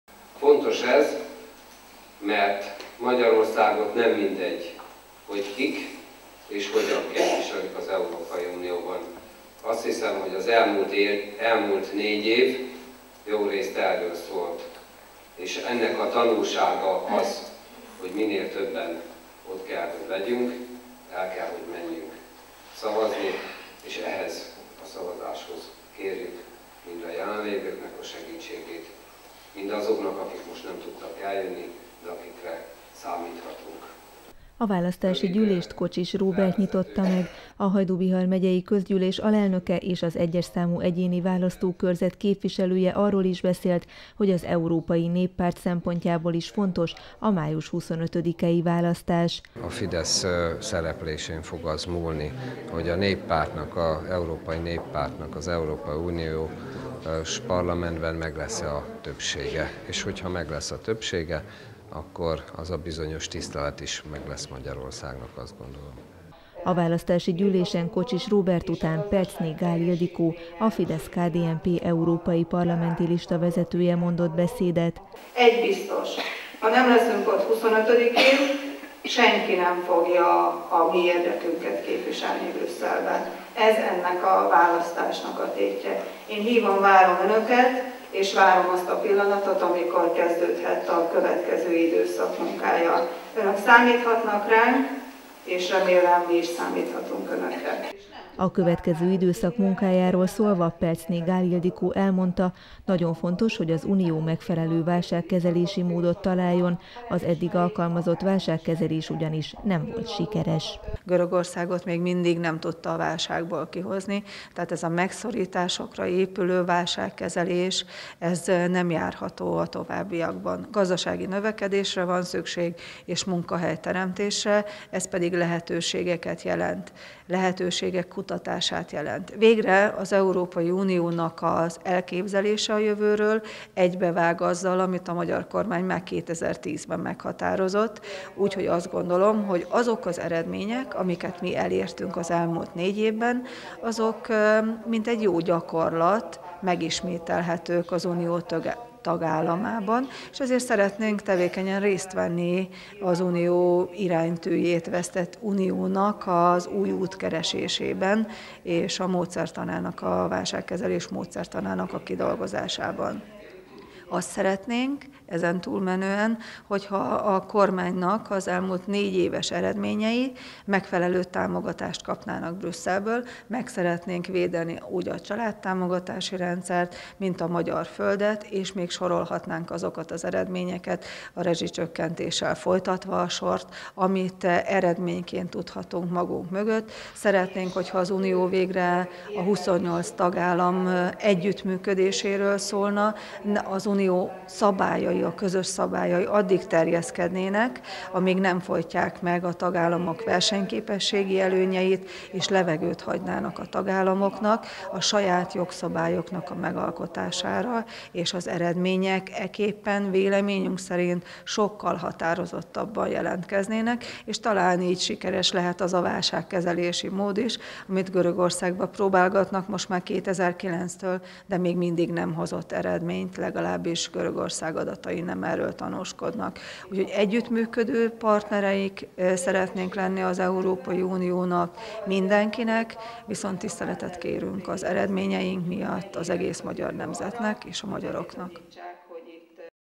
FIDESZ választási gyűlés
Komoly tétje van a május 25-i választásnak. Eldől, hogy hány olyan képviselő ül majd az Európai Parlamentben, aki Magyarország érdekeit képviseli. Ezt Pelczné Gáll Ildikó, a Fidesz-KDNP európai parlamenti listavezetője mondta Hajdúszoboszlón, a párt választási gyűlésén.